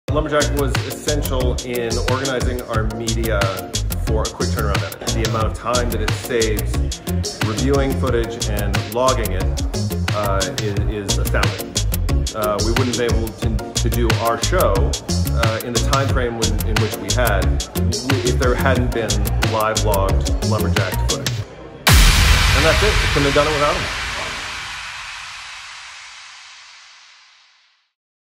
Geoffrey Orthwein Reviews Lumberjack
Director/Producer Geoffrey Orthwein depended on the Lumberjack System to organize media for the highly-rated film, "Bokeh."